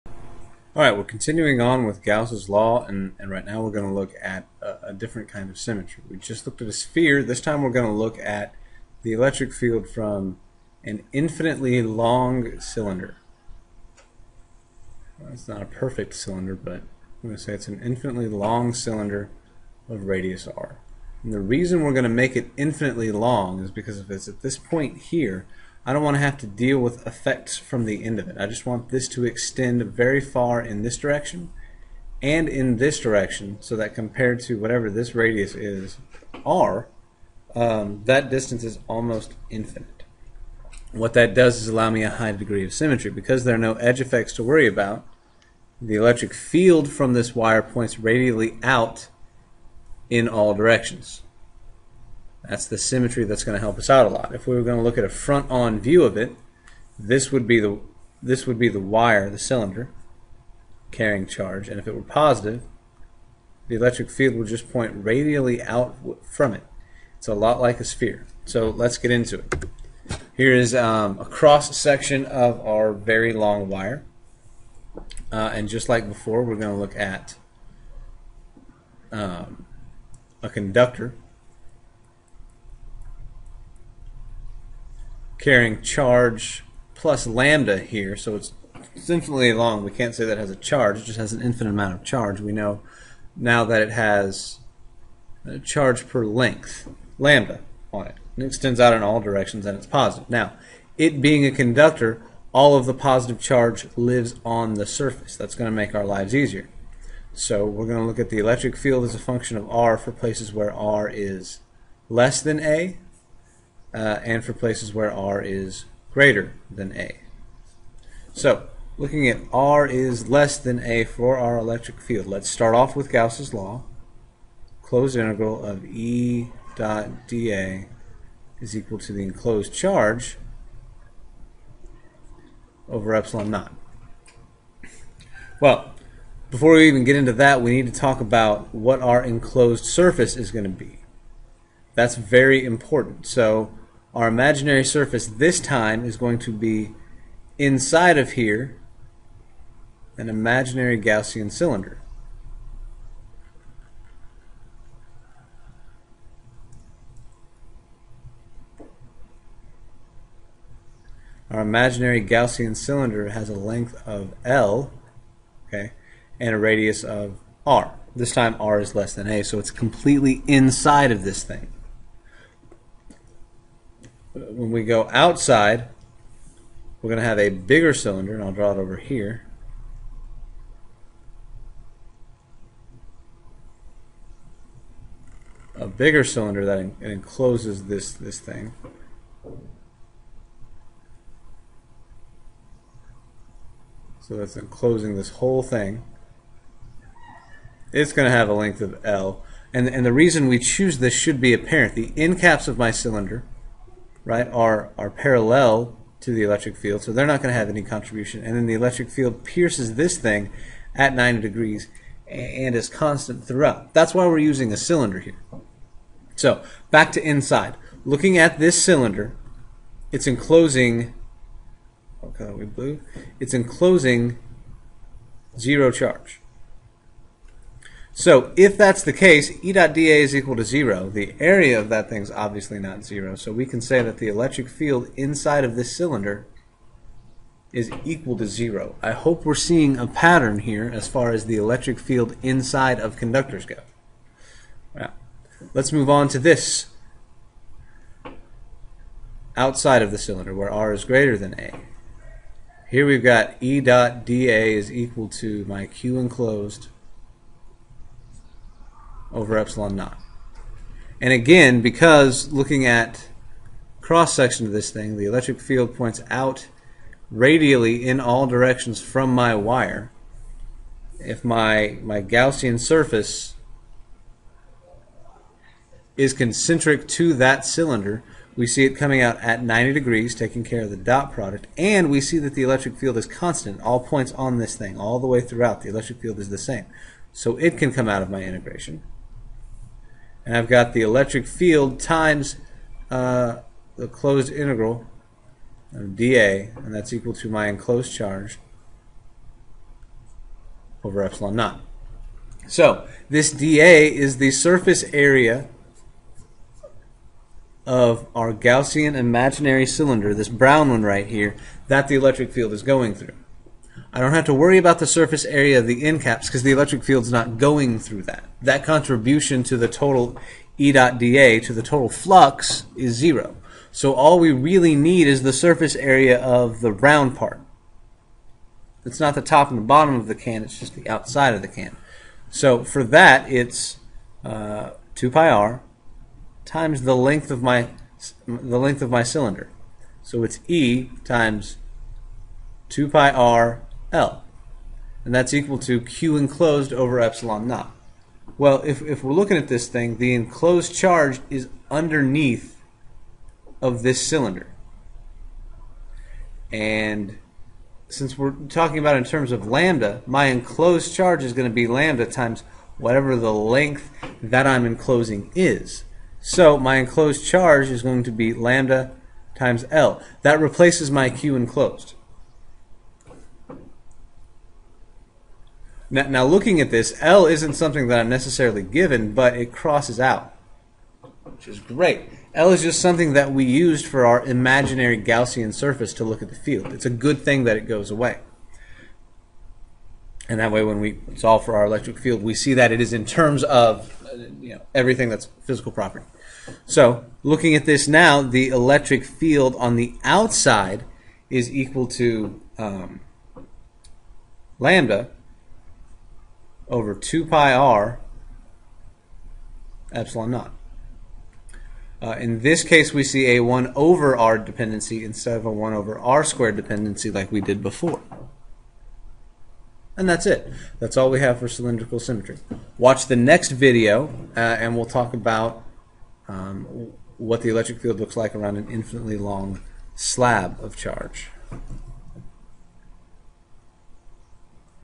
Gauss' Law 3
Finding the electring field of an infinitely long charged conducting cylinder.